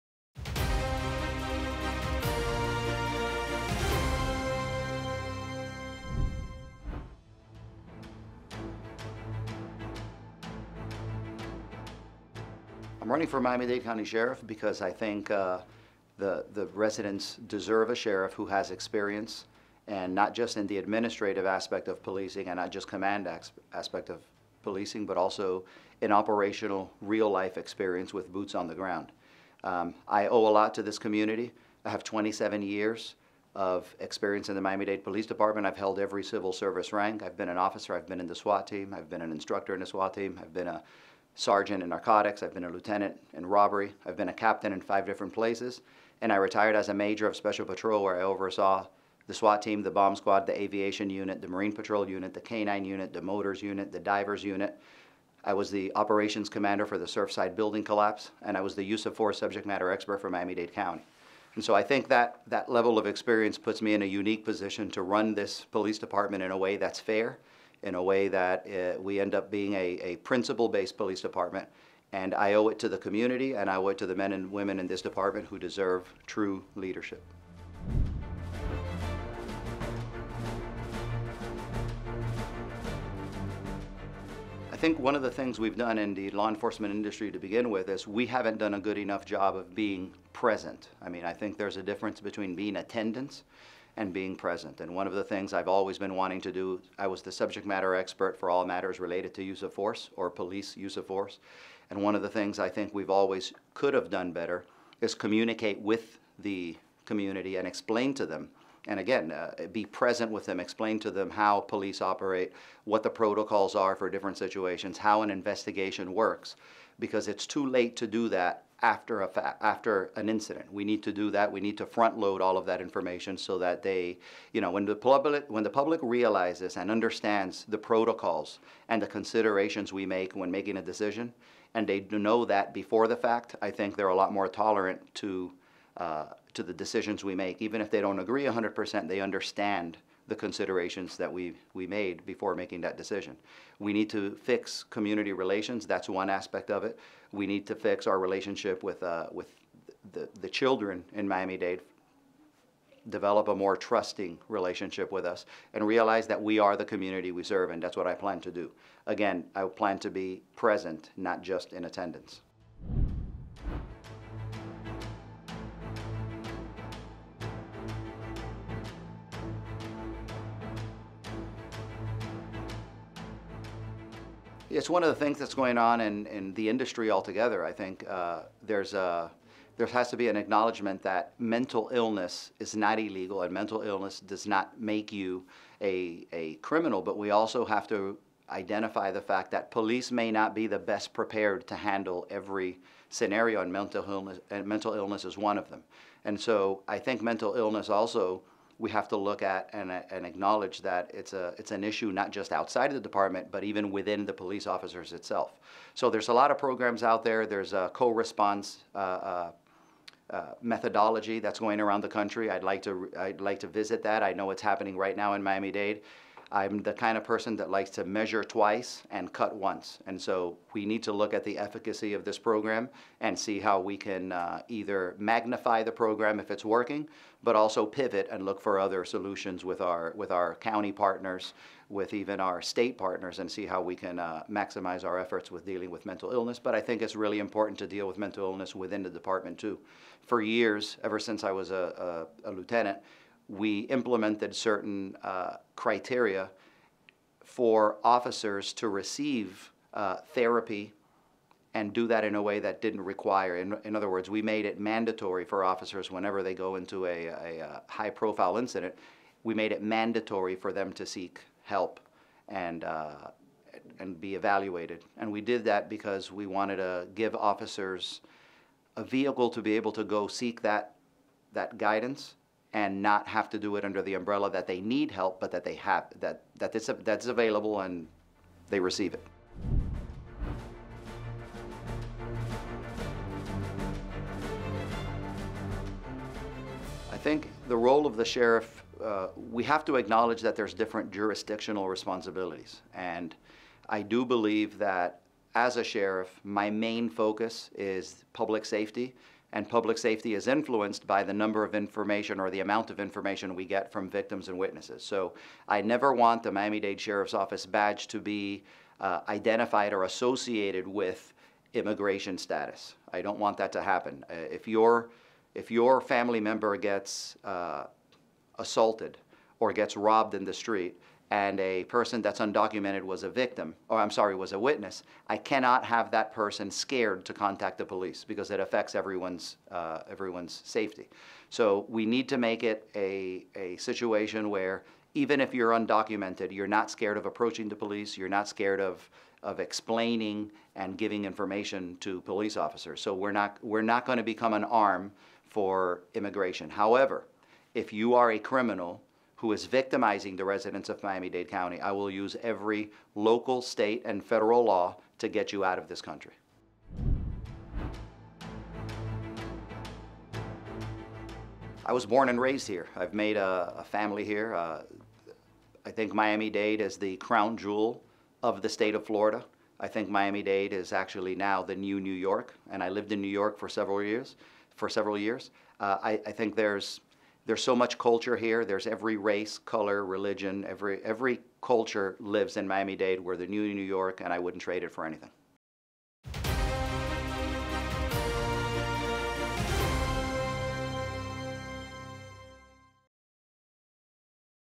CBS News Miami Interview with Mario Knapp
Republican Mario Knapp, 50, was born in Miami and lives in Miami-Dade County. He has 27 years of law enforcement experience.